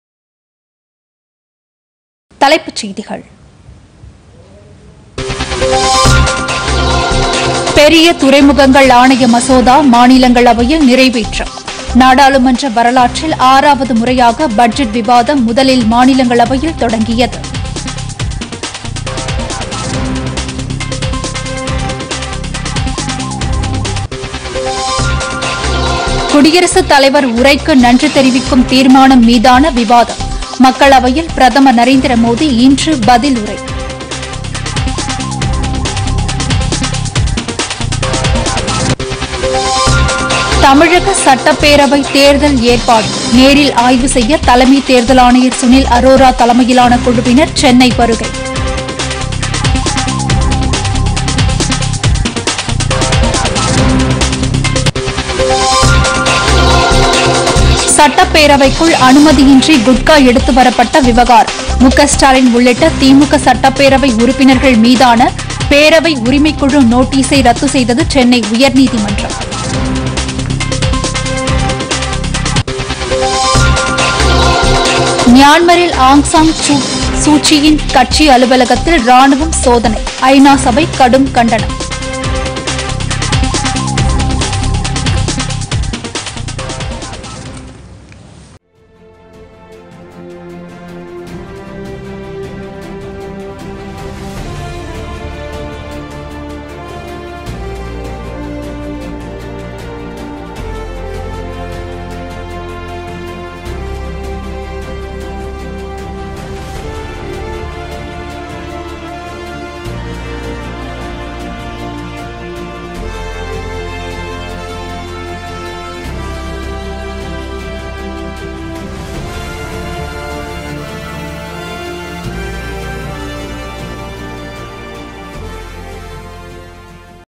பொதிகை பிற்பகல் 2.00 மணி தலைப்புச் செய்திகள் [10.02.2021]PodhigaiTamilNews
1)   பெரிய துறைமுகங்கள் ஆணைய மசோதா மாநிலங்களவையில் நிறைவேற்றம் -  நாடாளுமன்ற வரலாற்றில் ஆறாவது முறையாக பட்ஜெட் விவாதம் முதலில் மாநிலங்களவையில் தொடங்கியது.

2)  குடியரசுத் தலைவர் உரைக்கு நன்றிதெரிவிக்கும் தீர்மானம் மீதான விவாதம் - மக்களவையில் பிரதமர் நரேந்திர மோதி இன்று பதிலுரை.

3)  தமிழக சட்டப்பேரவைத் தேர்தல் ஏற்பாடு - நேரில் ஆய்வு செய்ய தலைமைத் தேர்தல் ஆணையர் சுனில் அரோரா தலைமையிலான குழுவினர் சென்னை வருகை.

4)  சட்டப்பேரவைக்குள் அனுமதியின்றி குட்கா எடுத்து வரப்பட்ட விவகாரம் - மு க ஸ்டாலின் உள்ளிட்ட திமுக சட்டப்பேரவை உறுப்பினர்கள் மீதான பேரவை உரிமைக்குழு நோட்டீஸை ரத்து செய்தது சென்னை உயர்நீதிமன்றம்.

5)  மியான்மரில்  ஆங் சாங் சூ-ச்சியின் கட்சி அலுவலகத்தில் ராணுவம் சோதனை - ஐநா சபை கடும் கண்டனம்.